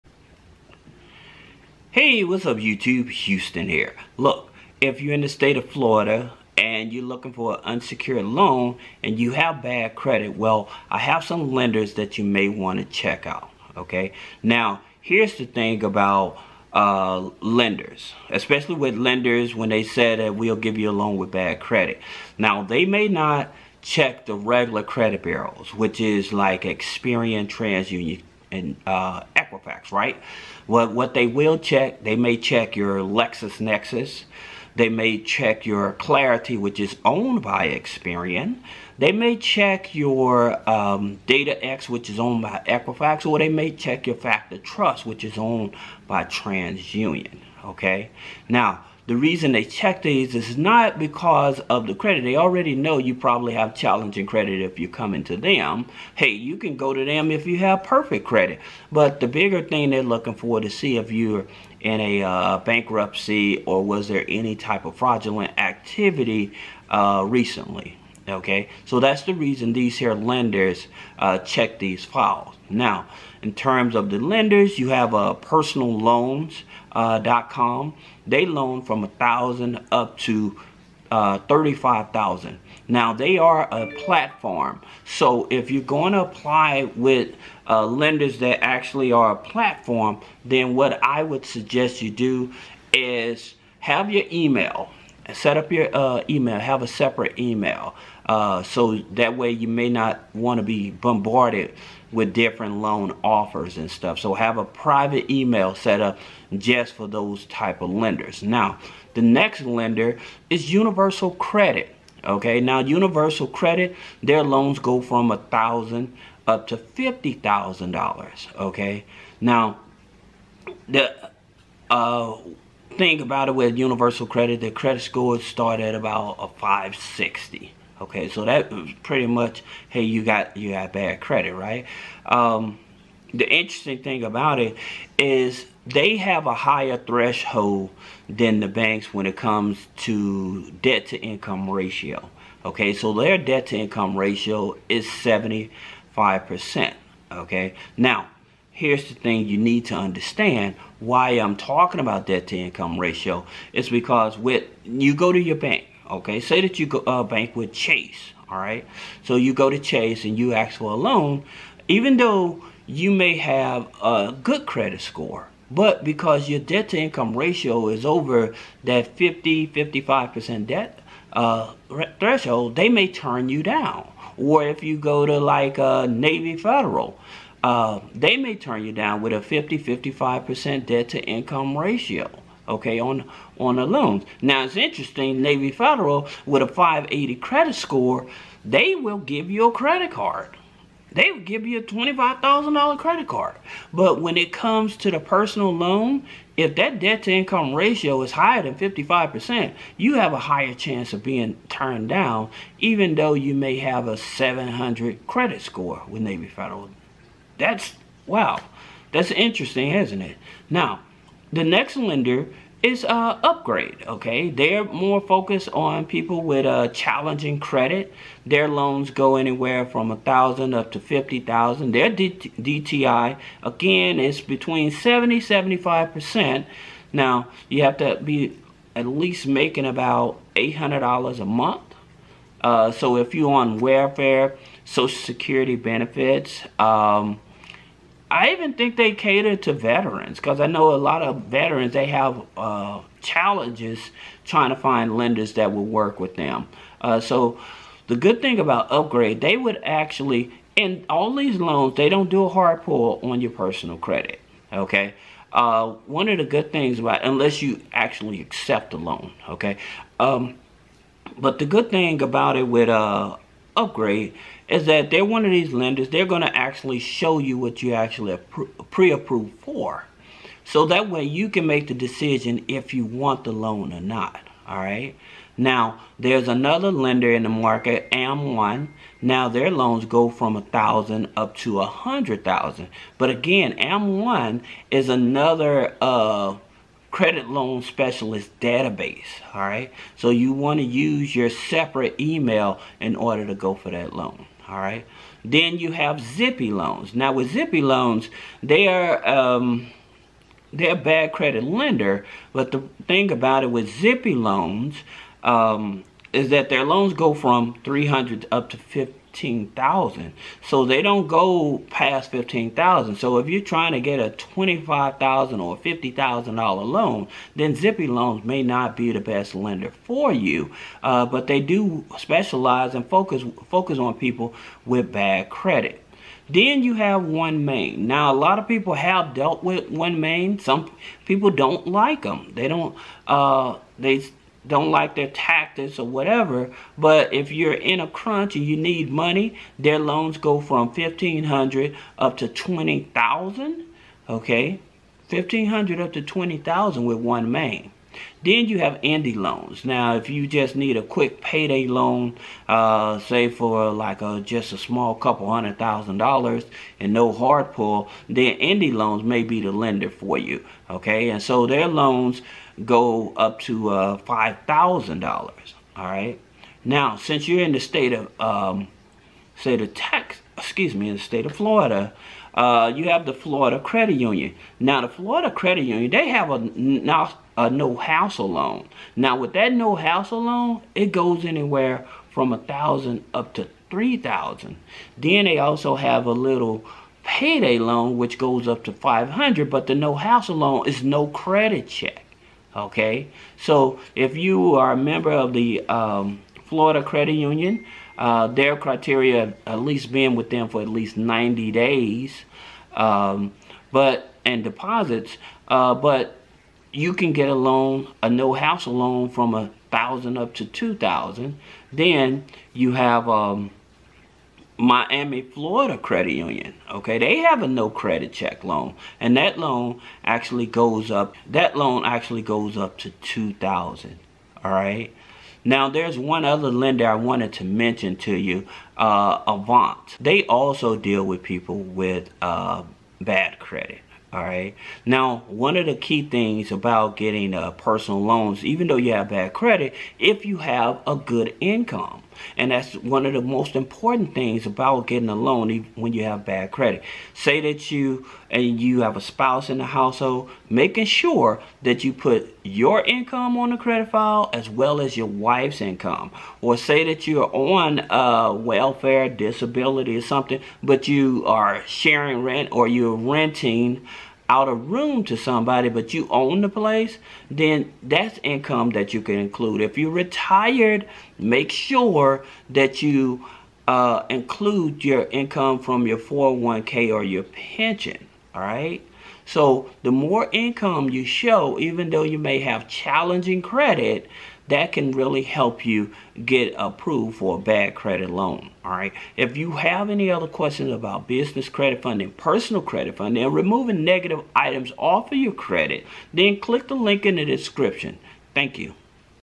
FLORIDA LOANS FOR BAD CREDIT 2023 | BEST 5 UNSECURED LOANS IN FLORIDA ONLINE
In today's video, we're going to be discussing 5 ways to increase your credit score fast without having to do any credit repair!

Credit scores are one of the most important factors when looking for a loan or signing up for a credit card. In this video, we'll be sharing with you some tips on how to raise your credit score fast without any credit repair. From paying your bills on time to maintaining a good credit history, these 5 tips will help you increase your credit score fast!

Houston Mcmiller, Business Credit America, Inc, or any of our associated websites, may be compensated for the links we share with the public audience.